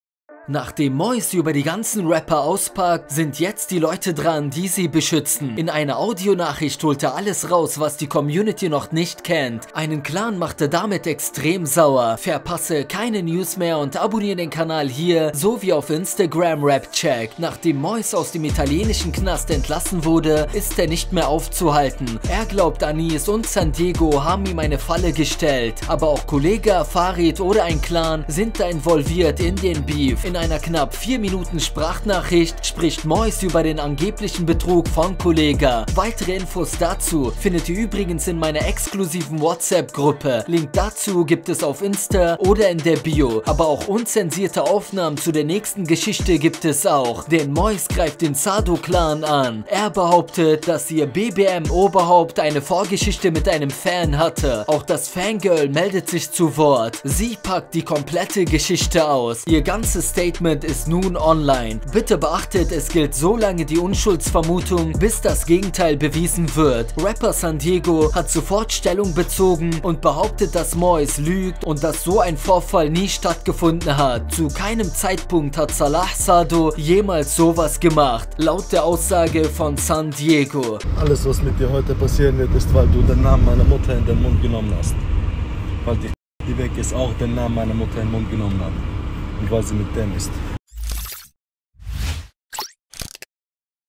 MOIS will den SAADO CLAN Exposen & diese Frau hilft ihm!
Quelle: Sun Diego, Kollegah, Mois